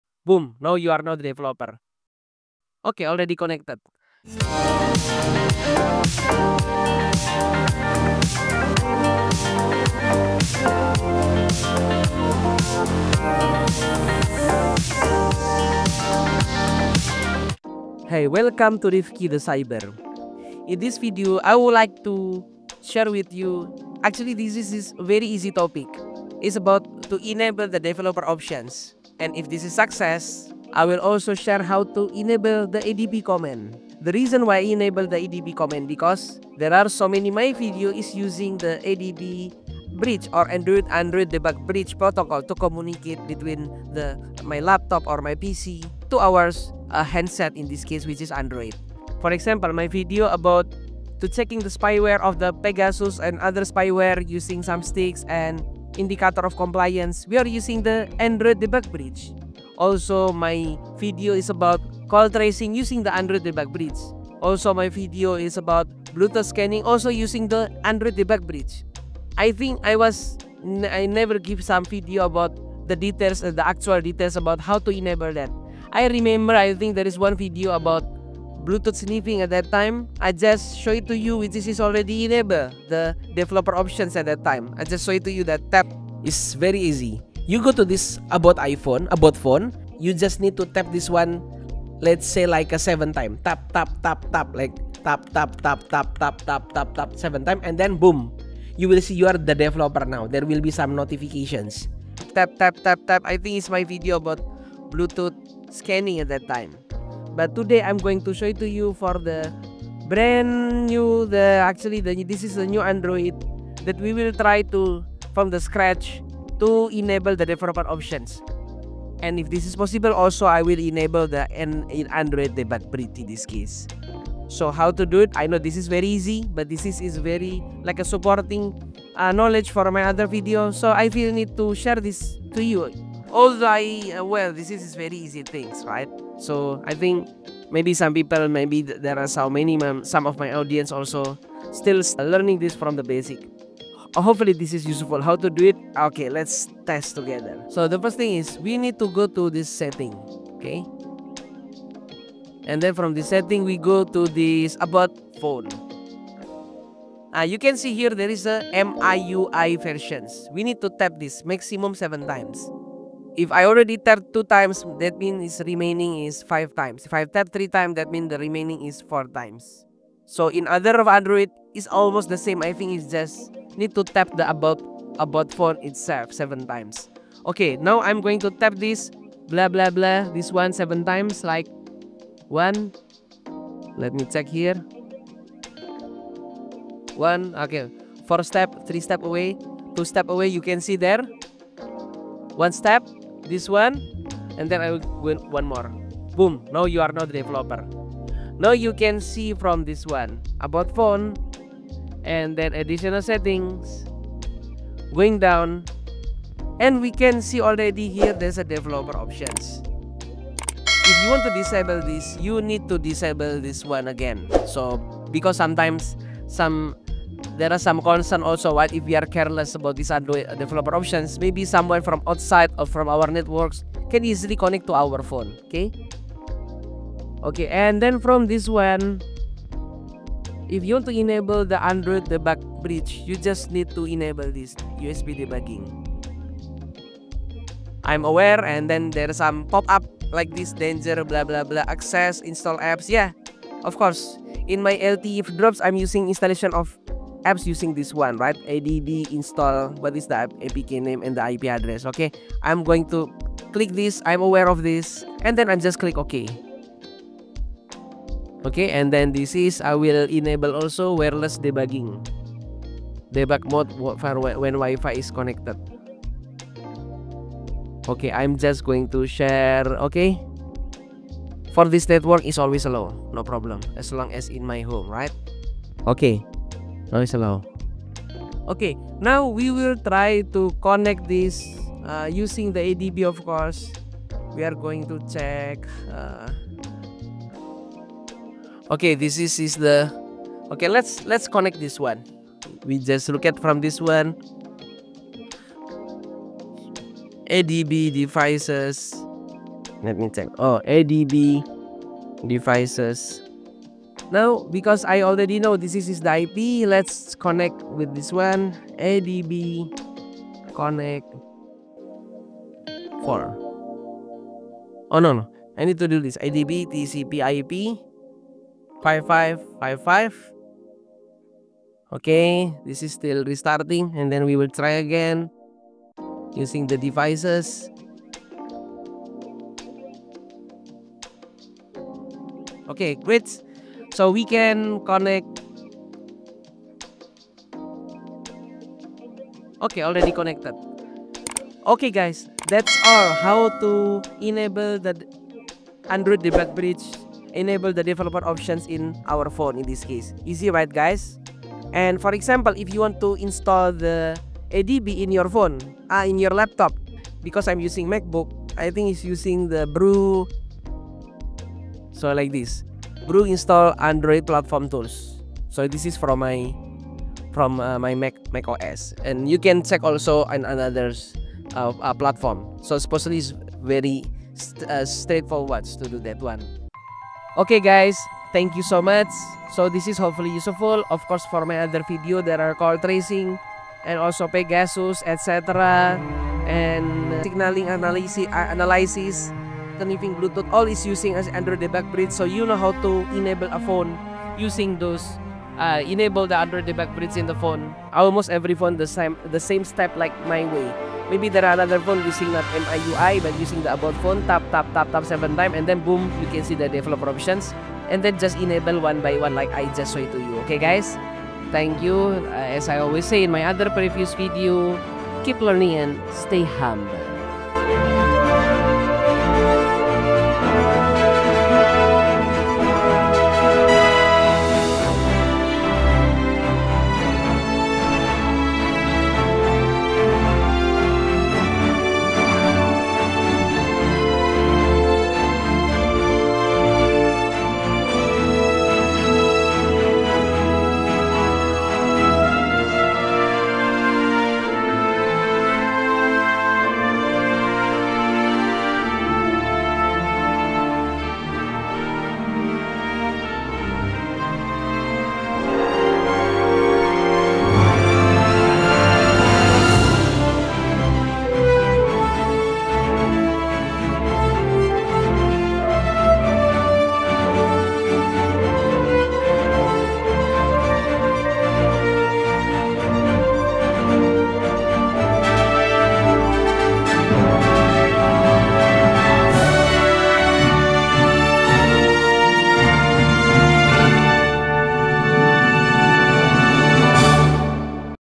How to Enable ADB And Usb Debugging on Android Phone
How to Enable USB Debugging  and Developer Options on Android Phone || ADB enable Tutorial.

ADB  (Android Debug Bridge): command-line tool that enables communication between a computer and an Android device

Below is the List of My Video that use or utilize the ADB command protocol: